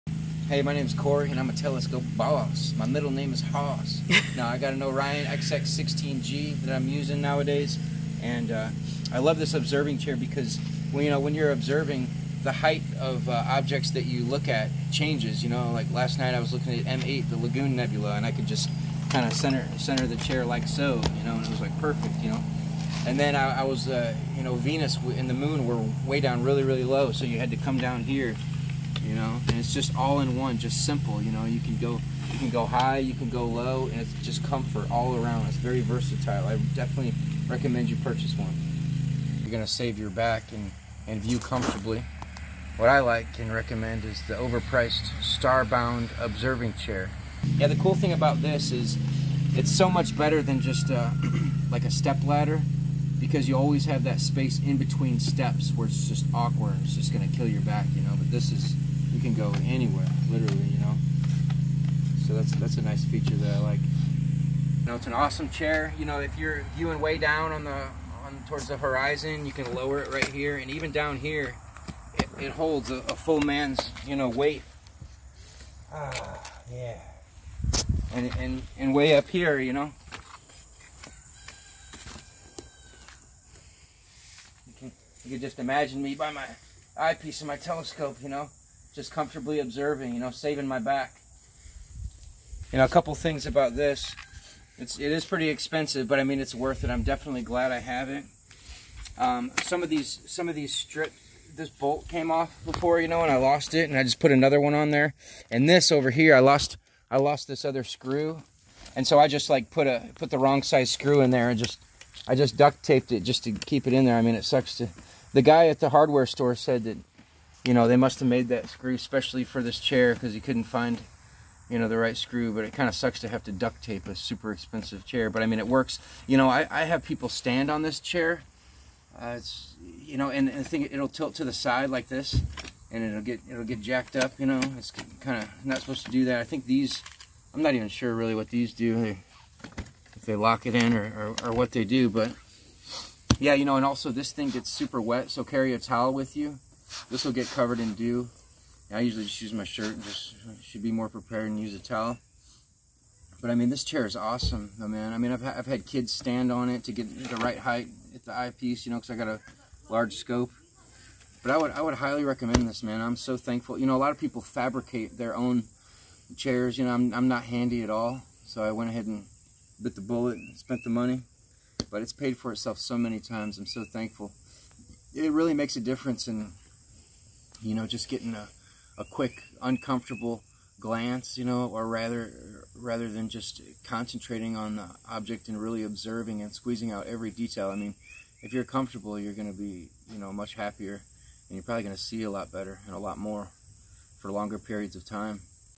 Do You Need an Observing Chair? Beginning Astronomy Setup
Do you need an observing chair with your first telescope?

I recommend one for every dob owner.

The bigger your telescope the more necessary it becomes.